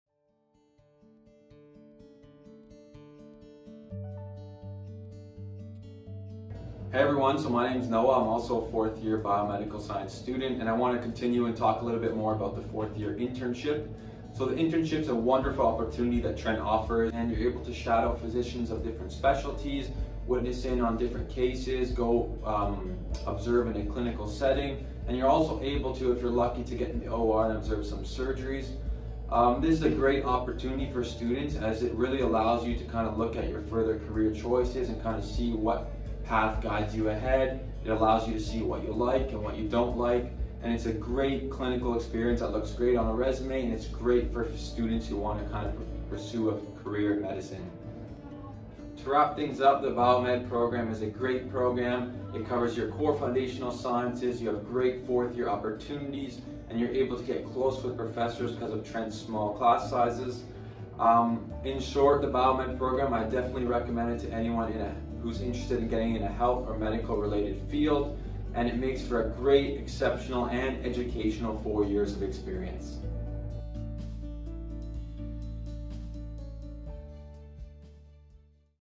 Biomedical Science - Medical Internships
A growing awareness of how the different levels of life interact has brought together the disciplines of biology and medicine and added to our understanding of how these interactions affect health and disease. Students who complete the Bachelor of Science (Honours) in Biomedical Science will appreciate that the many levels and approaches to health and medicine are complementary and will be able to apply these principles to assess and solve problems as they relate to real-world health issues.